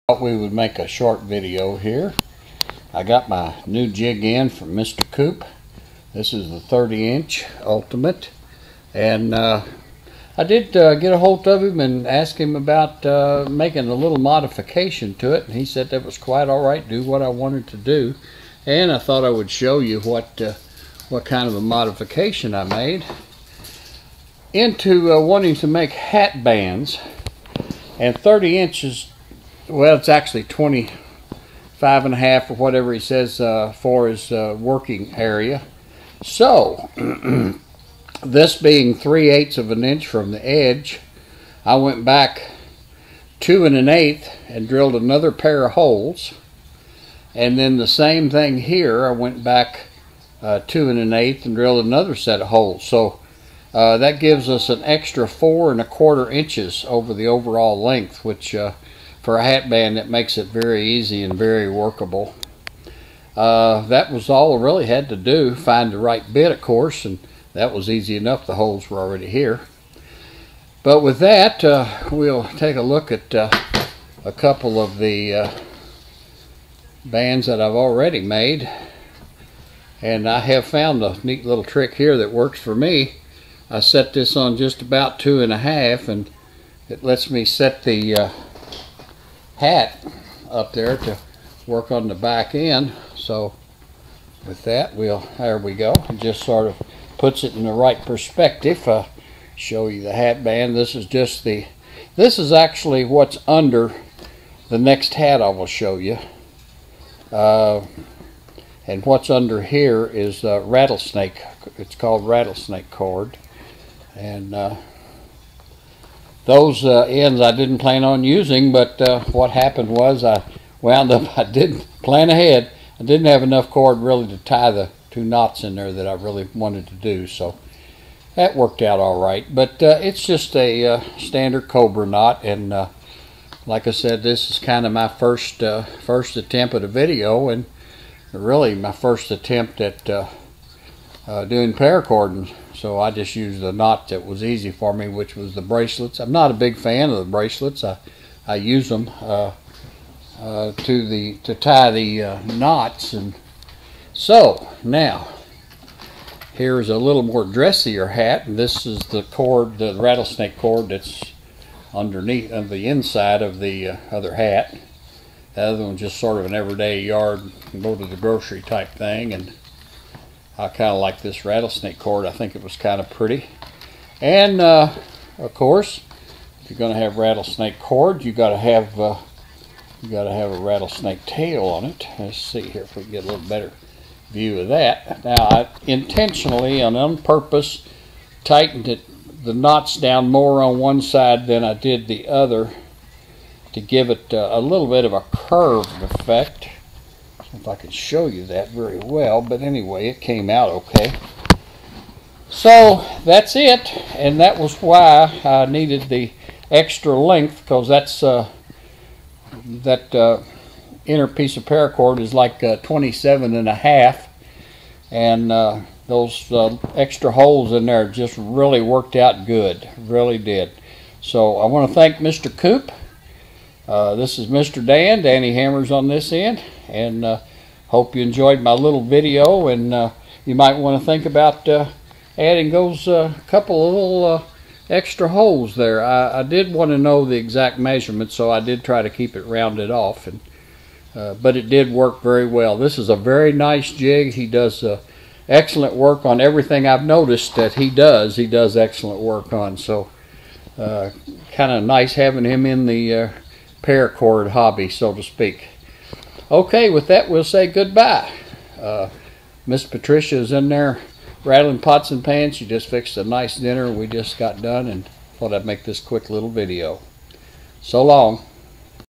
Dans Hat Jig
My new 30" jig from Mr. Coop and with a little mod I did. Hope you enjoy my little video, It's my first one and showing my Hat Bands. I really enjoy all of you all's work here in Swampville